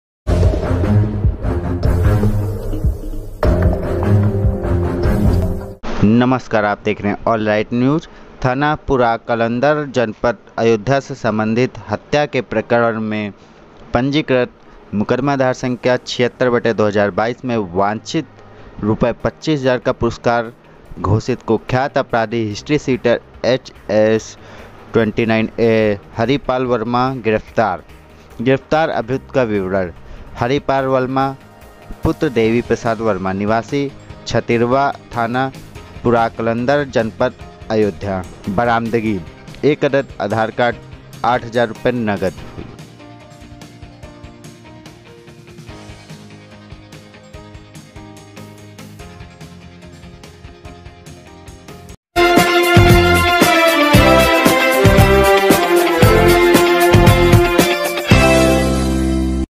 UP STF : आयोध्या से हत्या के प्रकरण में हिस्ट्रशीटर (एचएस-29ए) हरिपाल वर्मा गिरफ्तार
स्पेशल टास्क फोर्स, उत्तर प्रदेश, लखनऊ।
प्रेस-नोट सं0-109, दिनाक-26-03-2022
थाना पूराकलन्दर, जनपद आयोध्या से सम्बन्धित हत्या के प्रकरण में पंजीकृत मु0अ0सं0 76/2022 में वांछित, रू0 25,000/- का पुरस्कार घोषित कुख्यात अपराधी हिस्ट्रशीटर (एचएस-29ए) हरिपाल वर्मा गिरफ्तार
        दिनांक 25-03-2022 को एस0टी0एफ0, उत्तर प्रदेश को थाना पूराकलन्दर, जनपद अयोध्या में पंजीकृत मु0अ0सं0 76/2022 धारा 147/148/149/323/504/ 302/307/34/109 भादवि में वांछित, रू0 25,000/- का पुरस्कार घोषित कुख्यात अपराधी हरिपाल वर्मा को जनपद अयोध्या से गिरफ्तार करने में उल्लेखनीय सफलता प्राप्त हुई।

गिरफ्तार अभियुक्त का विवरणः

 हरिपाल वर्मा पुत्र देवी प्रसाद वर्मा निवासी छतिरवा, थाना पूराकलन्दर, जनपद अयोध्या।

बरामदगीः

 1- 01 अदद आधार कार्ड
      2- 8000 /- रू0 नगद
                                                                                                                गिरफ्तारी का स्थान, दिनांक/समय

 स्थान- डाभासेमर पुलिया की तरफ से सुलतानपुर रोड, थाना पूराकलन्दर, जनपद अयोध्या। दिनांक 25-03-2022, समय 22ः20 बजे।
  एस0टी0एफ0, उत्तर प्रदेश को विगत काफी दिनों से कुख्यात फरार/पुरस्कार घोषित अपराधियों के सक्रिय होकर हत्या करने व भयादोहन आदि अपराधों में लिप्त होने की सूचनायें प्राप्त हो रहीं थी। इस सम्बन्ध में एस0टी0एफ0 की विभिन्न इकाईयों/टीमों को अभिसूचना संकलन एवं कार्यवाही हेतु निर्देशित किया गया था, जिसके अनुक्रम में डा0 राकेश कुमार मिश्र अपर पुलिस अधीक्षक, एस0टी0एफ0, उ0प्र0 के पर्यवेक्षण में एस0टी0एफ0 मुख्यालय टीम द्वारा अभिसूचना संकलन की कार्यवाही की जा रही थी।
     अभिसूचना संकलन के दौरान ज्ञात हुआ कि थाना पूराकलन्दर, जनपद अयोध्या में पंजीकृत मु0अ0सं0 76/2022 धारा 147/148/149/323/504/302/307/34/109 भादवि में वांछित, रू0 25,000/- का पुरस्कार घोषित कुख्यात अपराधी हरिपाल वर्मा अपने गांव किसी काम से आया है। यदि शीघ्रता की जाये तो पकड़ा जा सकता है। इस सूचना पर उपनिरीक्षक श्री घनश्याम यादव के नेतृत्व में मु0आ0 प्रताप नारायण सिंह, मु0आ0 विनोद कुमार सिंह, कान्स0 राजेश, कान्स0 शिवभोला शुक्ला की एक टीम गठित कर मुखबिर द्वारा बताये गये स्थान से उपरोक्त अभियुक्त को आवश्यक बल प्रयोग कर गिरफ्तार कर लिया गया, जिससे उपरोक्त बरामदगी हुई।
 गिरफ्तार अभियुक्त ने पूछताछ में बताया कि उसने अपने मित्रों के साथ जमीनी रंजिश के कारण परेशान होकर दिनांक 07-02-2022 को ग्राम छतिरवा मंे सूर्य प्रकाश दुबे उर्फ सूरज तथा उनके भतीजे अखण्ड प्रताप दुबे उर्फ शक्ति दुबे की हत्या करने की नीयत से क्वालिस गाड़ी से उनकी मोटर साइकिल मे टक्कर मार कर उन लोगों को खाई में गिरा दिया था। उसके बाद हथौड़े व डण्डे से पीट-पीट कर मार डाला था तथा शक्ति दुबे भाग कर बच निकला। हम लोग उसी क्वालिस गाड़ी से भाग गये थे। रास्ते में जनता के द्वारा घिर जाने पर जनता के लोगों द्वारा मौके से दिनेश कुमार यादव व राम कुमार वर्मा को पकड़ लिया गया था। घटना के बाद भागते समय मेरे द्वारा हत्या में प्रयुक्त हथोड़ी को रास्ते में छुपा दिया गया था। भागने के बाद मुझे पता चला कि मेरे ऊपर पुलिस द्वारा इनाम घोषित कर दिया गया है, जिसके कारण मैं अलग-अलग जनपदों में छिप कर रह रहा था। आज अपनी पत्नी से मिलने आया था कि आप लोगों द्वारा पकड़ लिया गया।
गिरफ्तार अभियुक्त हरिपाल वर्मा का ज्ञात आपराधिक इतिहासः-

क्रं0 मु0अ0सं0 धारा थाना जनपद
1 332/1992 392/294 भादवि व 3(1)10 एससी/एसटी एक्ट पूराकलन्दर अयोध्या
2 313/1994 307 भादवि पूराकलन्दर अयोध्या
3 65/1995 307 भादवि पूराकलन्दर अयोध्या
4 655/1997 332/323/504/506 भादवि पूराकलन्दर अयोध्या
5 212/1994 504/506/352  पूराकलन्दर अयोध्या
6 94/1998 3/4 गुण्डा एक्ट  पूराकलन्दर अयोध्या
7 1198/2003 147/148/149/302/307 भादवि, 7 सीएलए एक्ट व 3(1) यू0पी0 गैंगेस्टर एक्ट  कोतवाली नगर अयोध्या
8 76/2022 47/148/149/323/504/302/
307/34/109 भादवि पूराकलन्दर अयोध्या
एच0एस0 संख्याः 29ए

गिरफ्तार अभियुक्त को थाना पुराकलन्दर, जनपद अयोगध्या में पंजीकृत उपरोक्त अभियोग में दाखिल किया गया है। अग्रेतर विधिक कार्यवाही स्थानीय पुलिस द्वारा की जा रही है।